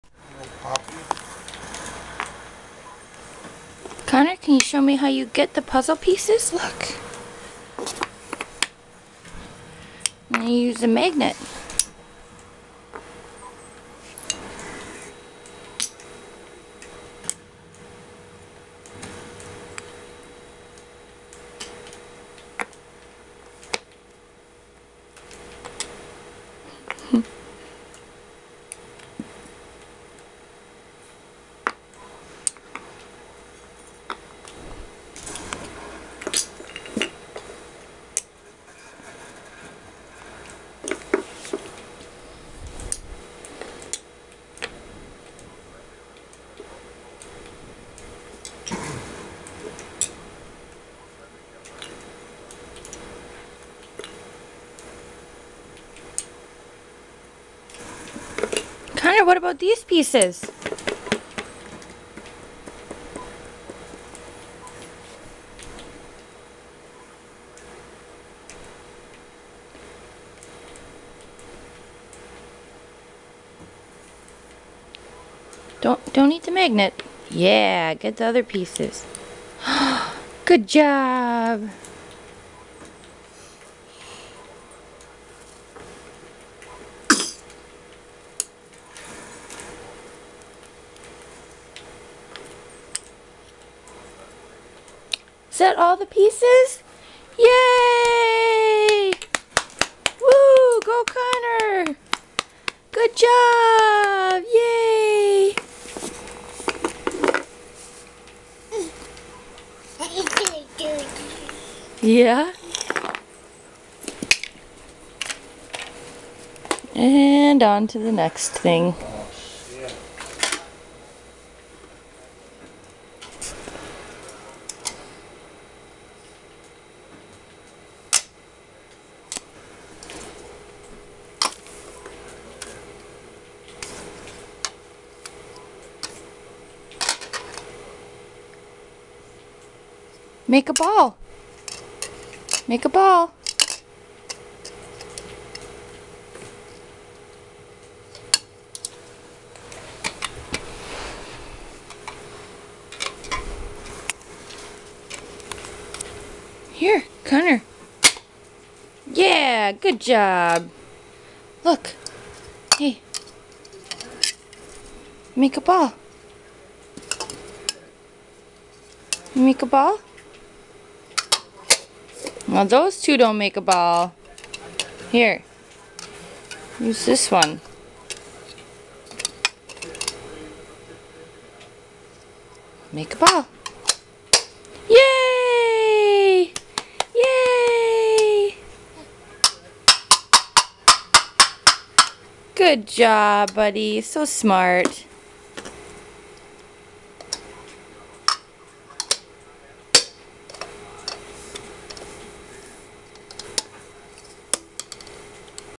Connor using magnetic puzzle
One year old son, playing with a magnetic puzzle and some stacking cups. He uses the magnet to grab the pieces so well!